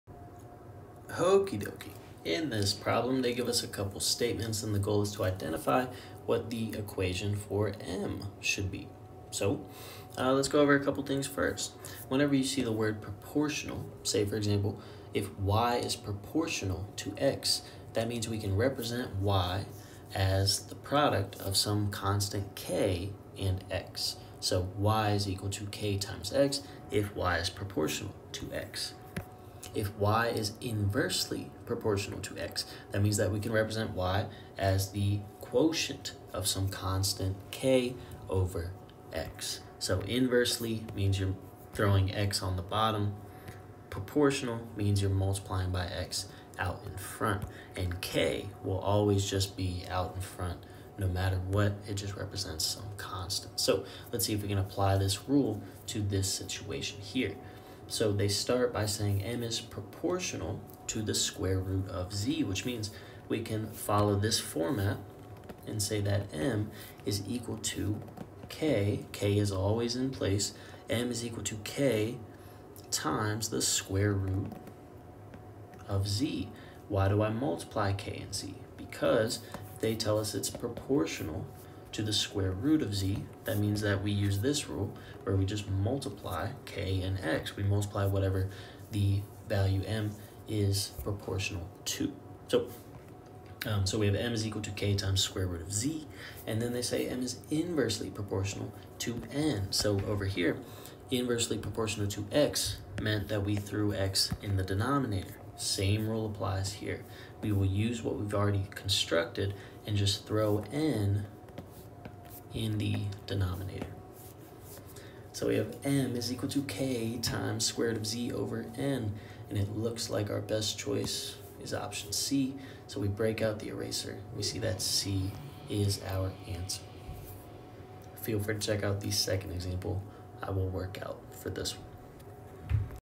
Quiz 2 problem 1.1.2a (VT MATH 1026)
Timestamps:
00:00 Hokie dokie
00:09 Directly vs inversely proportional (k is a constant)
00:58 Statement 1
01:35 Statement 2
01:50 Use the statements to create a single equation